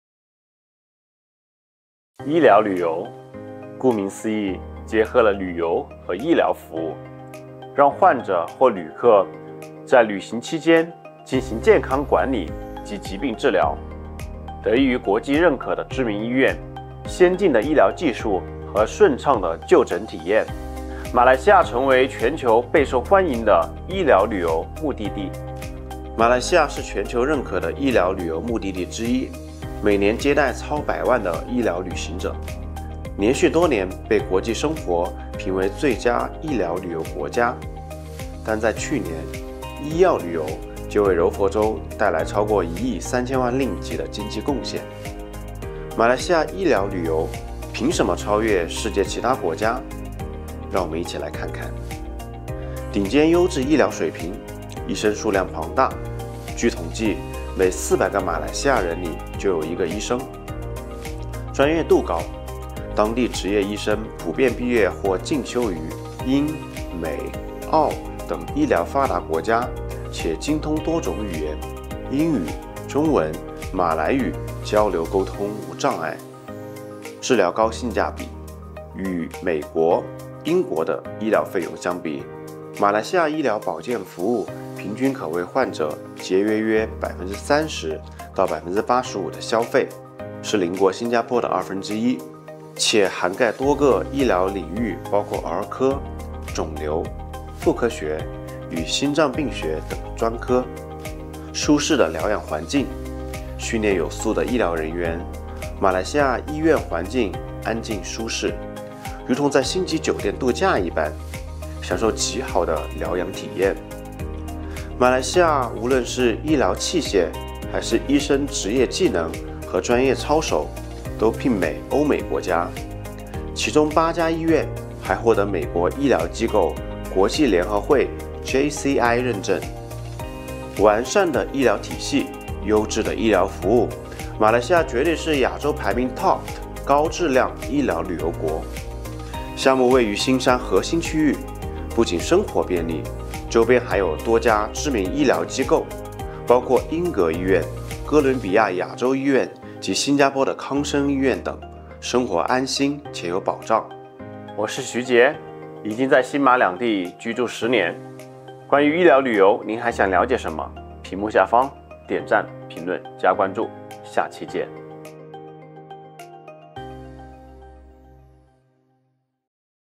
【R&F Princess Cove 富力公主湾】Malaysia is a global leader in medical tourism 马来西亚是全球认可的医疗旅游目的地之一
Malaysia is a global leader in medical tourism with top-notch healthcare, affordability, and a comprehensive system. Malaysia is undoubtedly a top-ranked destination for high-quality medical tourism in Asia. R&FPrincessCove in Johor Bahru's heart is near leading hospitals like Gleneagles Hospital, Columbia Asia Hospital and Thomson Medical Centre, offering a secure and convenient lifestyle.

马来西亚是全球认可的医疗旅游目的地之一。顶尖医疗水平➕完善的医疗体系及优质的医疗服务，堪称亚洲排名TOP的高质量医疗旅游国。
富力公主湾 位于新山核心区域，不仅生活便利，周边还有多家知名医疗机构，包括鹰阁医院、哥伦比亚亚洲医院及新加坡的康生医院等，生活安心且有保障！